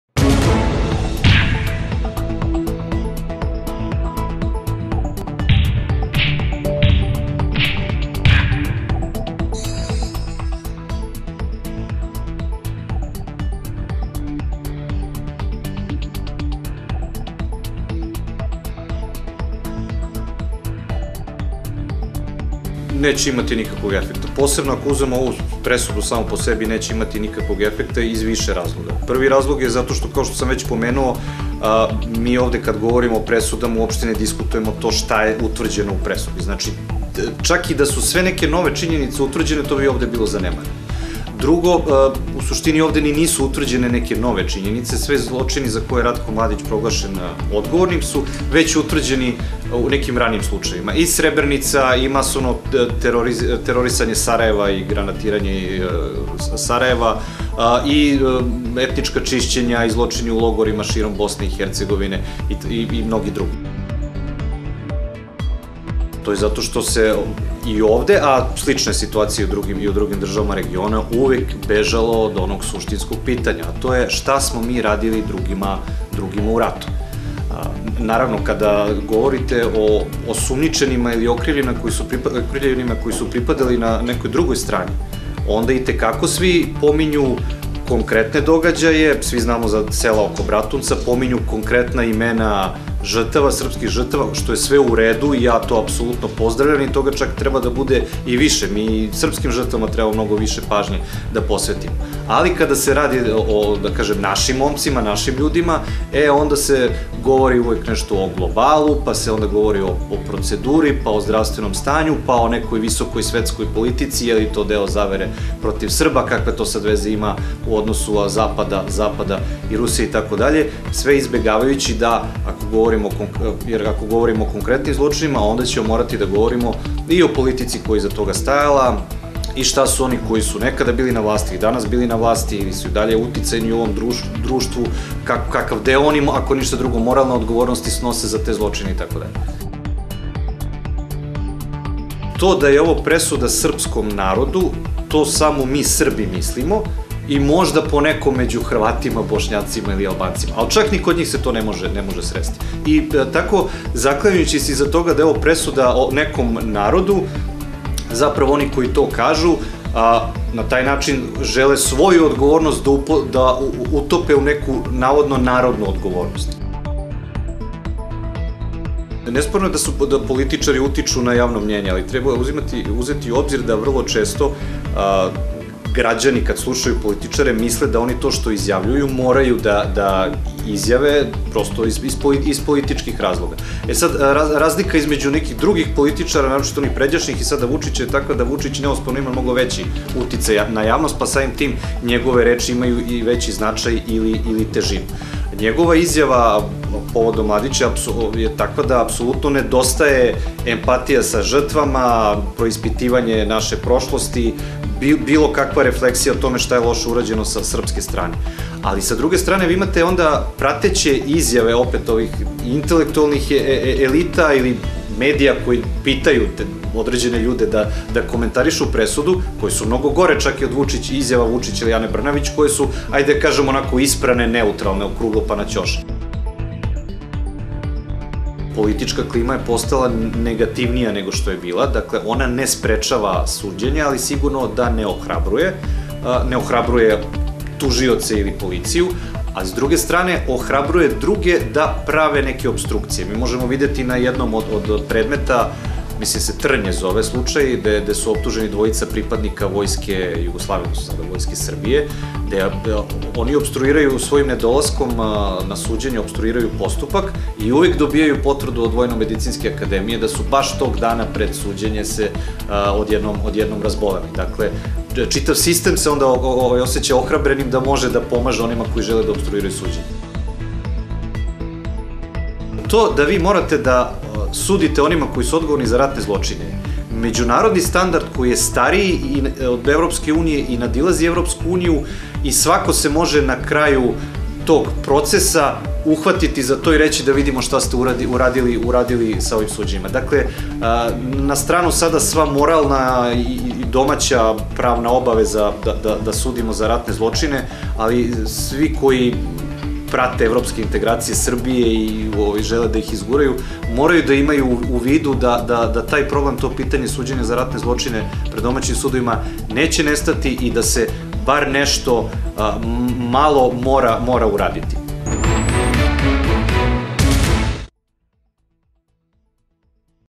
Pomirenje zadatak elita
Haški tribunal je apsolutno prevazišao sva očekivanja, izjavio FoNetu ekspert za međunarodno krivično pravo Ivan Jovanović i ocenio da je zadatak Suda bio da utvrdi krivičnu odgovornost konkretnih ljudi, dok je pomirenje "posao političkih elita i kod nas i u regionu, koji one ne rade".